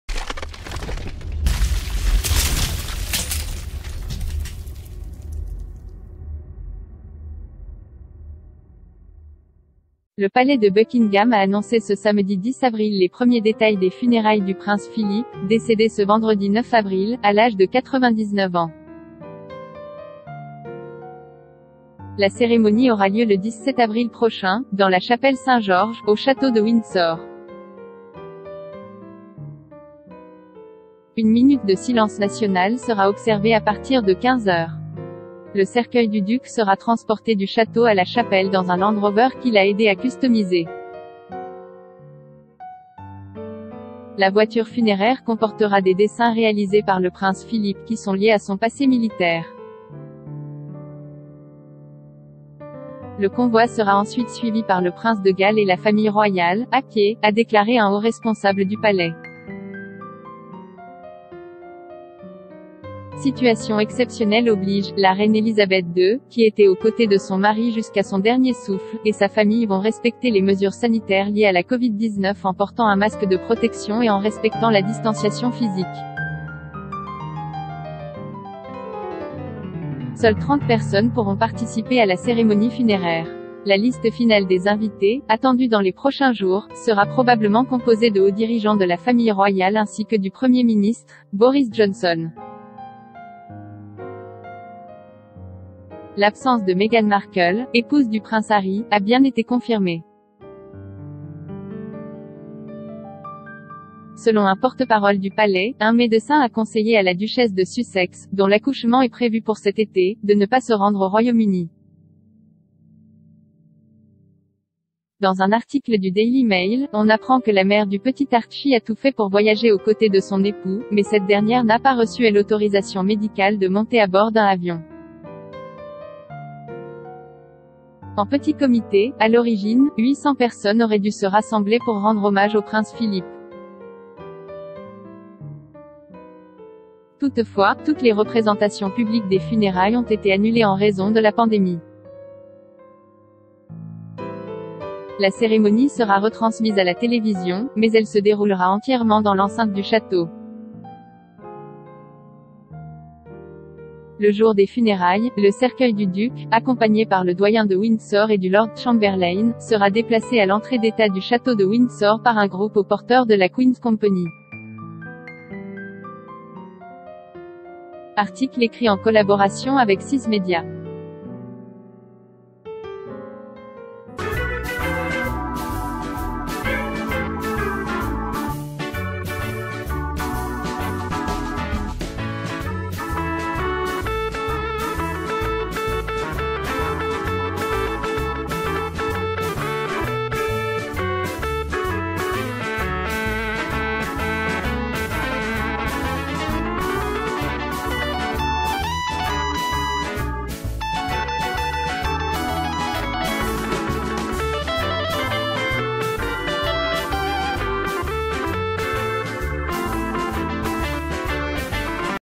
✅ Funérailles du prince Philip : date, chapelle privée, voiture customisée… Les détails dévoilés
✍ Le palais de Buckingham a annoncé ce samedi 10 avril les premiers détails des funérailles du prince Philip, décédé ce vendredi 9 avril, à l'âge de 99 ans.  La cérémonie aura lieu le 17 avril prochain, dans la chapelle St-Georges, au château de Windsor.  Une minute de silence nationale sera observée à partir de 15 heures. Le cercueil du duc sera transporté du château à la chapelle dans un Land Rover qu'il a aidé à customiser.  La voiture funéraire comportera des dessins réalisés par le prince Philip qui sont liés à son passé militaire.  Le convoi sera ensuite suivi par le prince de Galles et la famille royale, à pied, a déclaré un haut responsable du palais.  Situation exceptionnelle oblige, la reine Elizabeth II, qui était aux côtés de son mari jusqu'à son dernier souffle, et sa famille vont respecter les mesures sanitaires liées à la Covid-19 en portant un masque de protection et en respectant la distanciation physique.  .Seules 30 personnes pourront participer à la cérémonie funérair ...